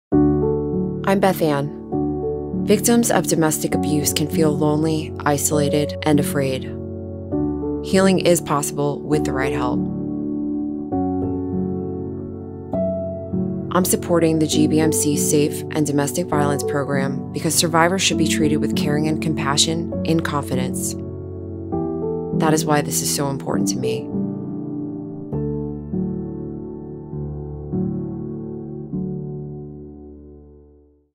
What It Means to Me
"Victims of Domestic Violence and abuse can feel lonely, isolated, and afraid. Healing is possible with the right help."

We collaborated with Beth-Ann Wilson, owner of Night Owl Gallery in Baltimore, to paint a butterfly to have donated to the GBMC Sexual Assault Forensic Examination Unit (SAFE) and Domestic Violence Unit, to share the symbol of hope, change and perseverance.

Take a stand. Support victims of abuse and domestic violence